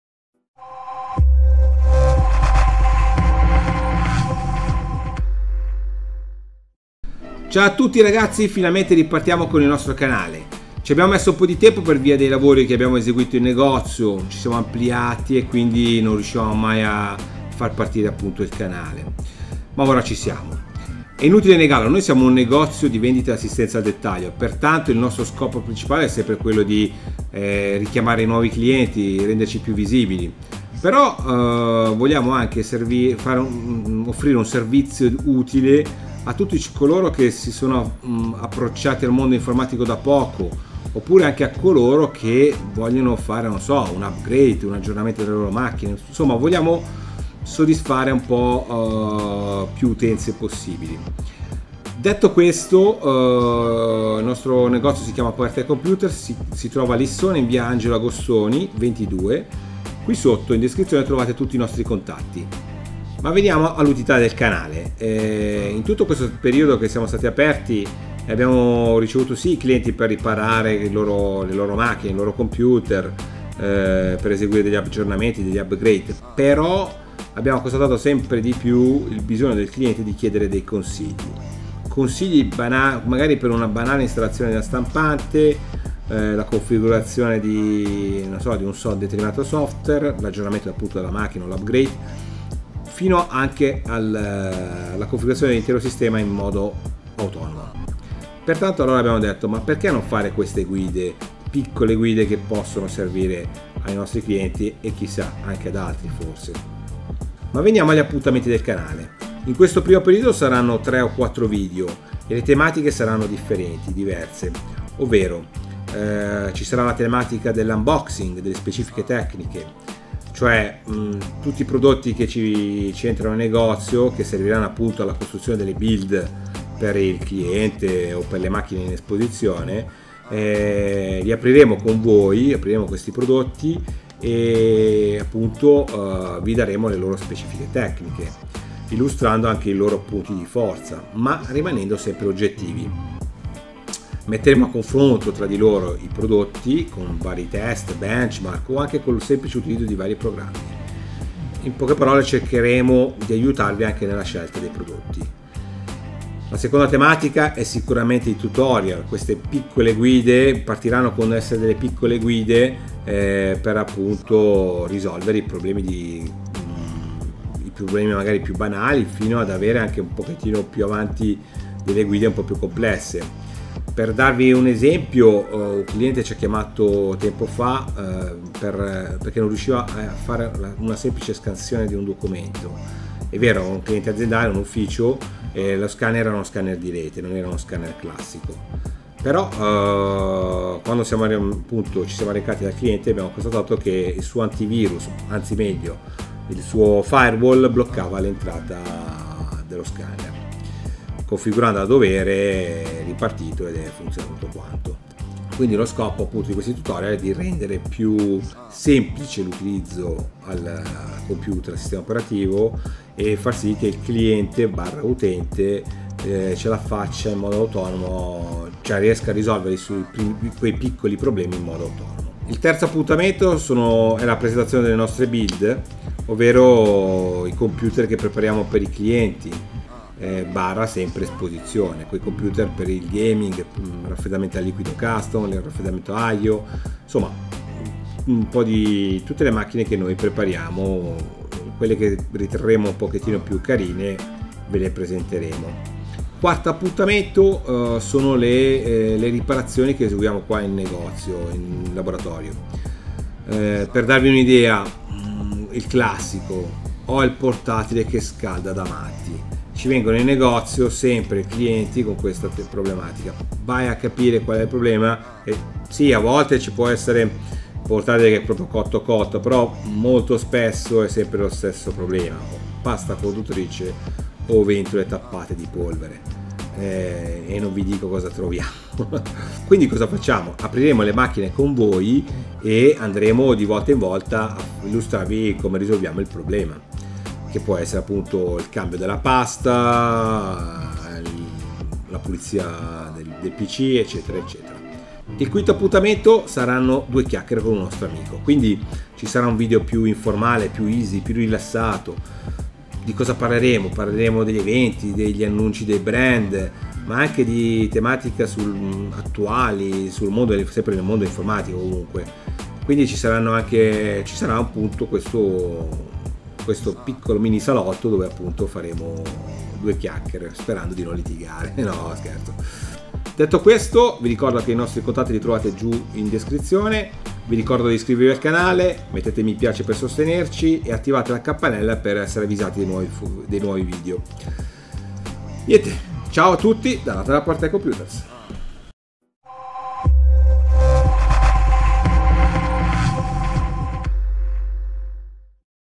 Presentazione Canale
Presentazione dei contenuti che caricheremo sul canale. Varie tematiche:
1 - "Unboxing e Test Hardware" , testeremo i prodotti che arrivano in negozio e li metteremo a confronto tra di loro con Benchmark o su utilizzo software quotidiano.
2 - "Tutorial Software" , mini guide di come utilizzare i vari software, dal sistema operativo a quelli di produttività, dai software di sicurezza come antivirus e firewall a quelli di svago, ecc...
3 - "Le nostre Build" : Vi presenteremo i pc e soluzioni che adottiamo per i nostri clienti, PC Gaming; Raffreddamento Liquido Aio o Custom, ecc...
4 - "Riparazioni" , caricheremo sul canale i video di riparazioni o di upgrade dei pc che i nostri clienti ci portano in laboratorio.
5 - PwTech Talk Shaw : Due chiacchiere in negozio su eventi, fiere o news riguardante il mondo informatico.